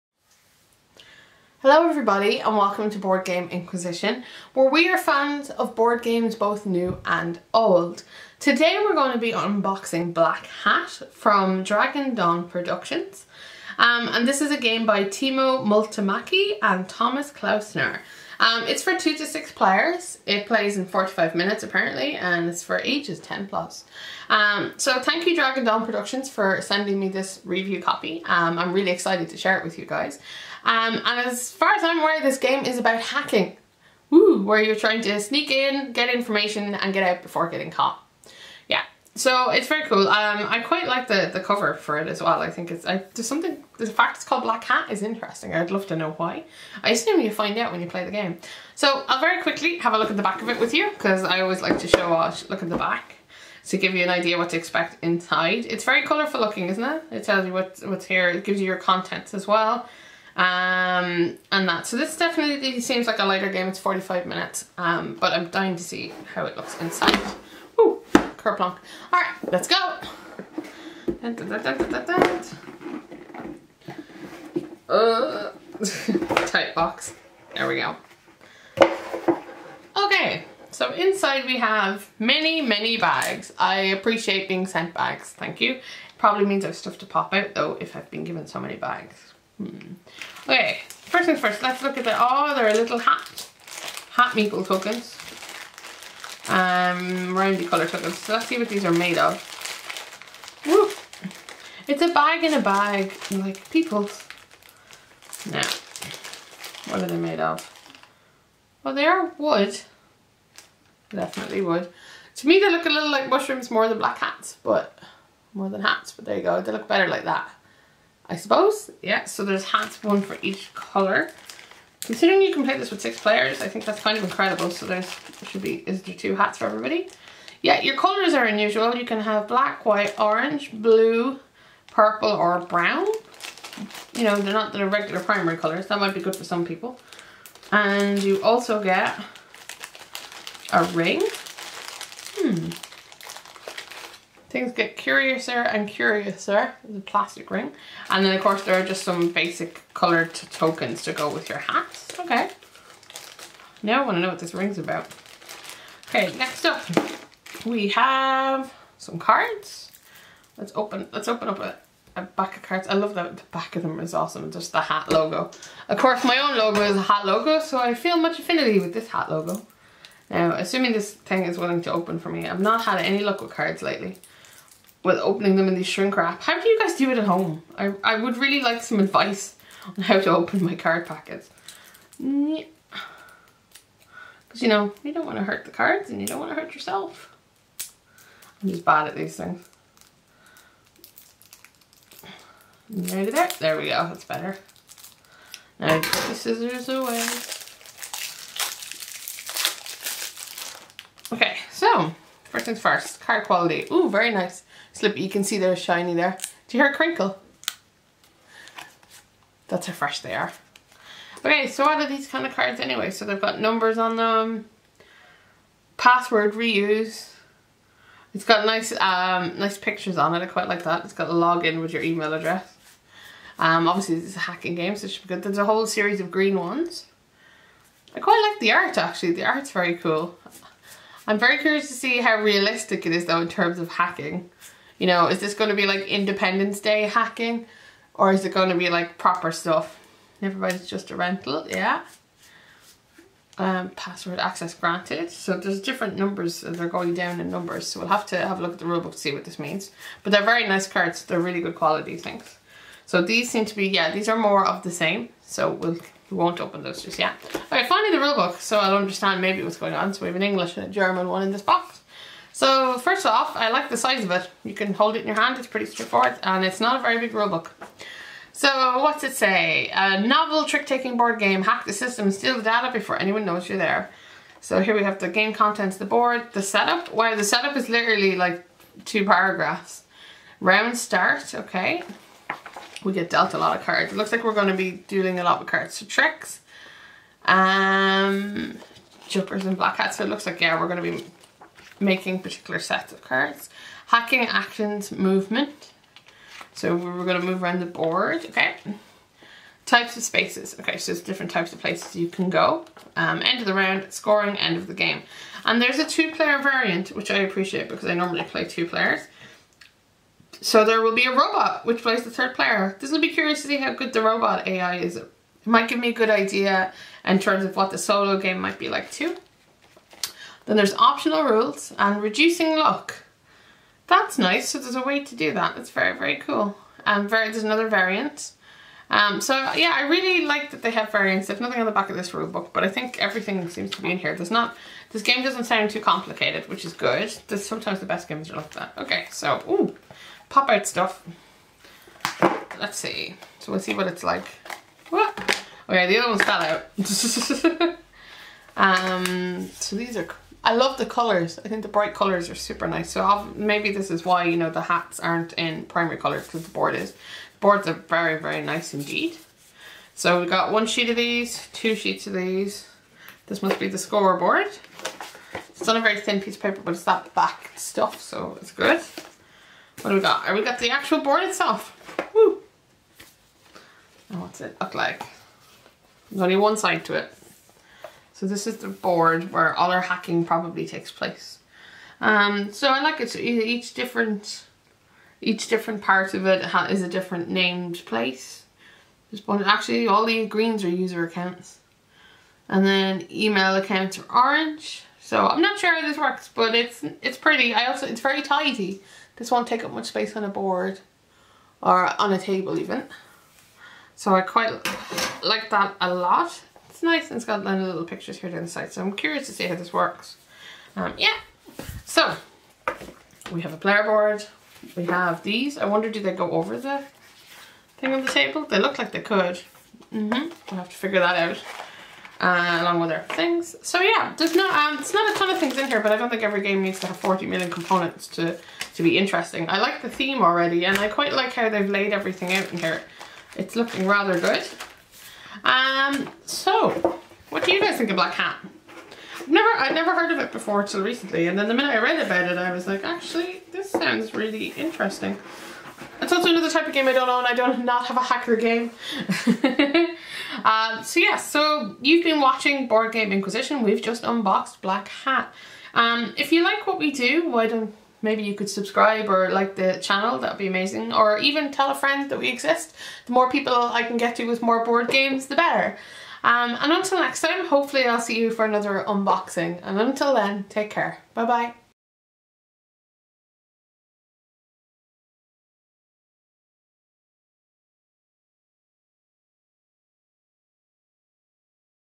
Black Hat Unboxing
In this video, Board Game Inquisition unboxes Black Hat from Dragon Dawn Productions.